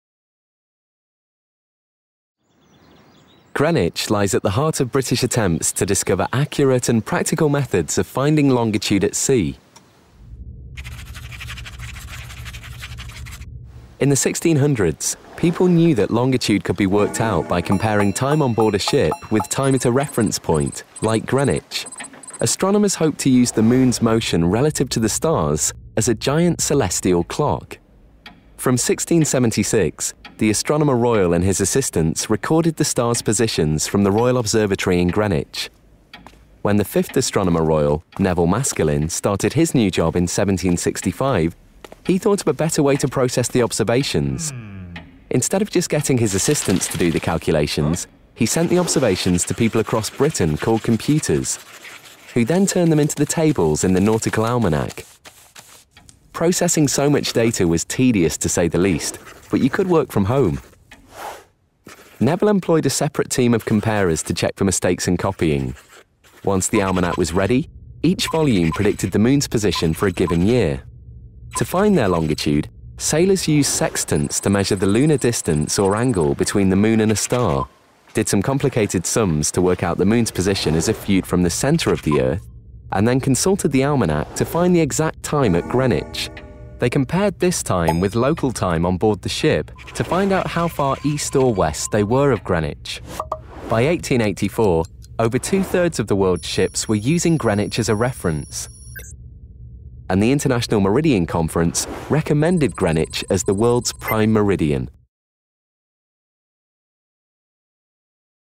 Making Greenwich the centre of the world
Cambridge University Library and the National Maritime Museum have digitised the Board of Longitude archive.  from 18th July 2013.

This is the final film in the series and tells the story of how the Nautical Almanac was produced thanks to work of a band of human computers. This publication helped sailors find their position at sea.